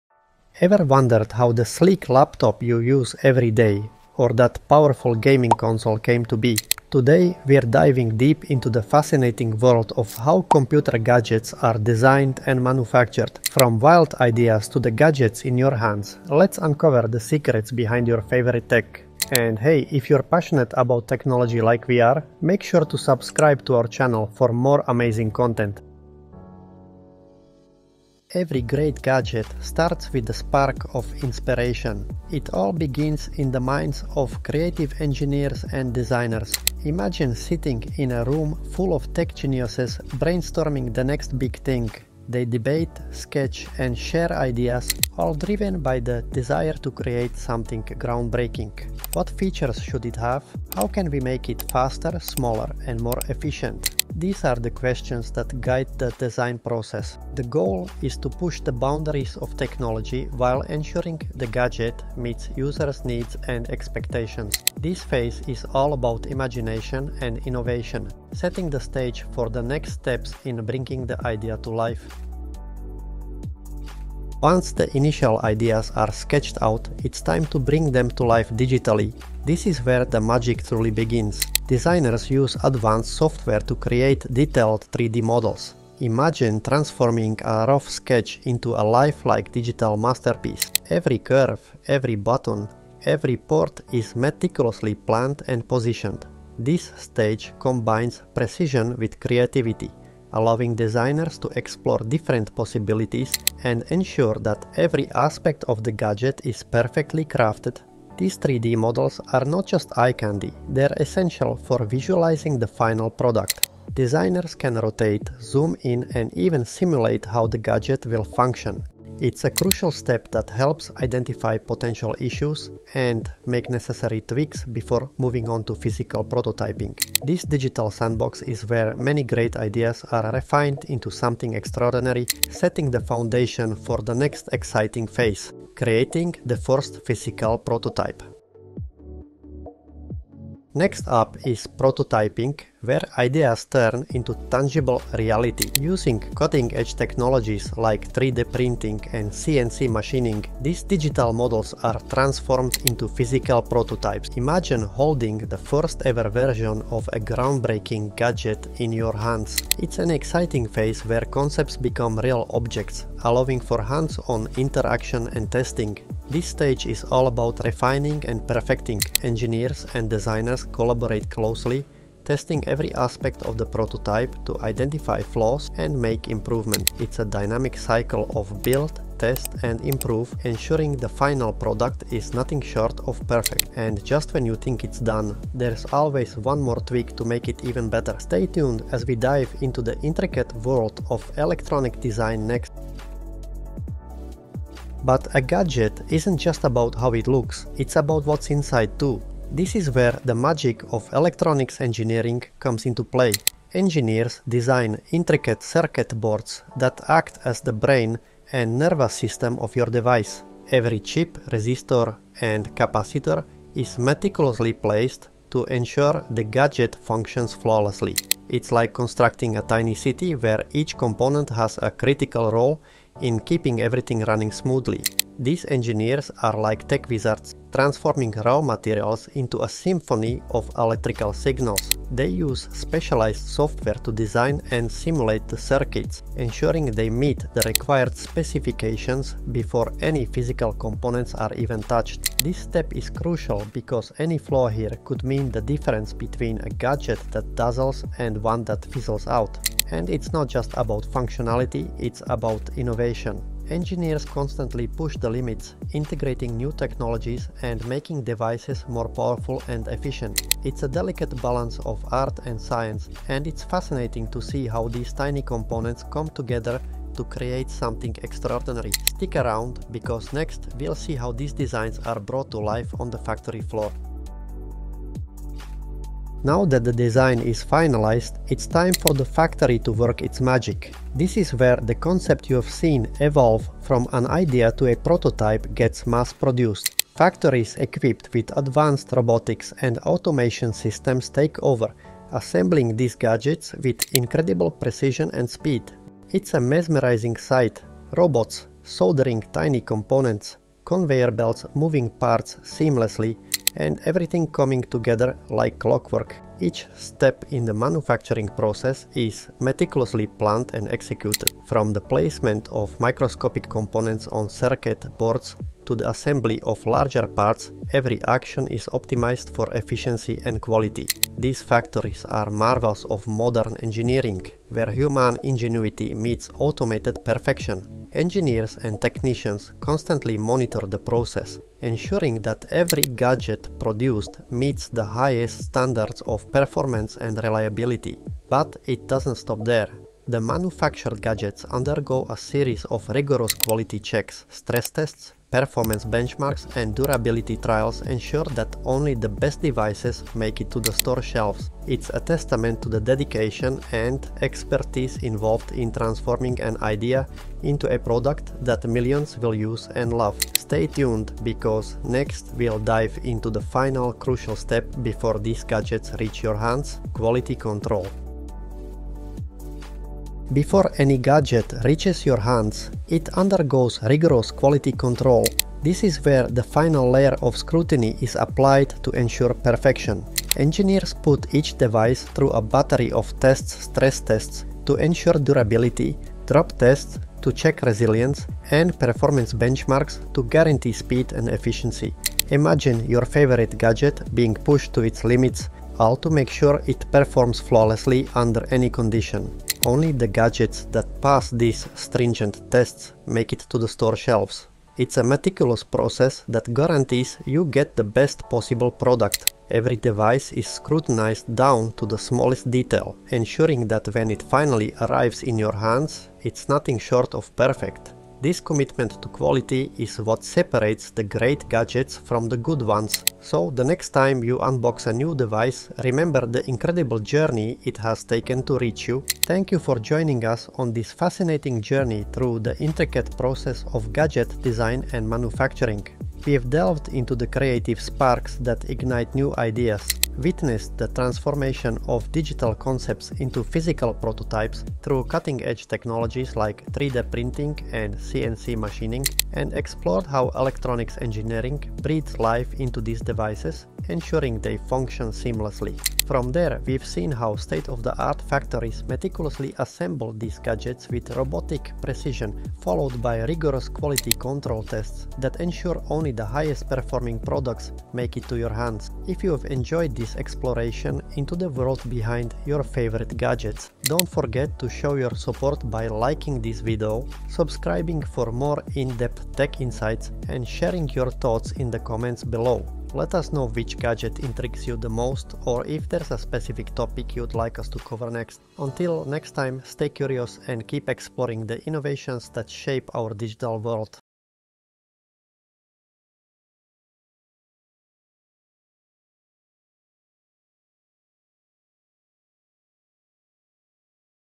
How Computer Gadgets Are Designed and Manufactured ⚙️
"How Computer Gadgets Are Designed and Manufactured" is a video that explores the process behind the creation of computer gadgets. It covers the entire journey from initial design concepts to final production, showcasing the technologies and techniques used in each stage. Viewers get an inside look at the design processes, prototyping, material selection, manufacturing methods, and quality control measures that ensure the creation of high-quality gadgets.

Yours CMD Tech
NSE4, NSE7, CCNP, CCNA Security, CCNA Wifi, CCNA R+S